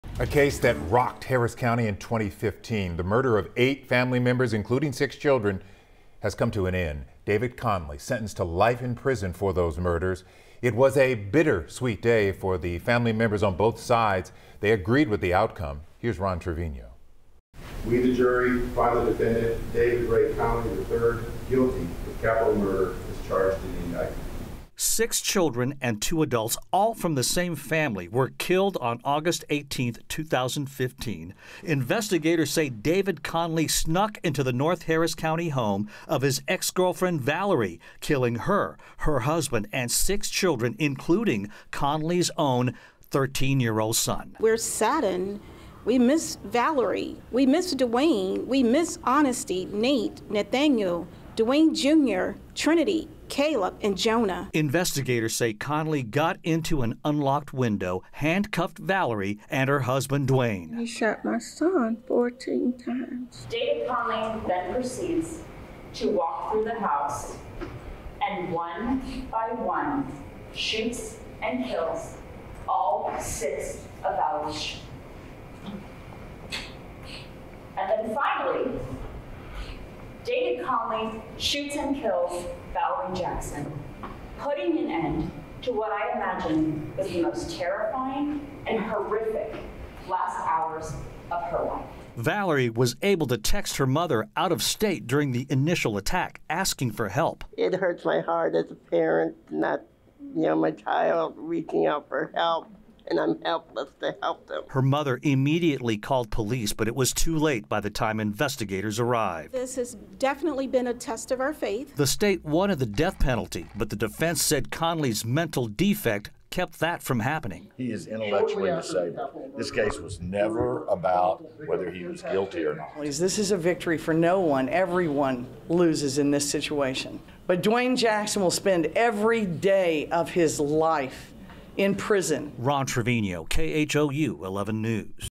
David Conley sentenced to life in prison after killing family of 8
David Conley has been sentenced to life in prison after killing eight people, including six children -- one of them being his own kid.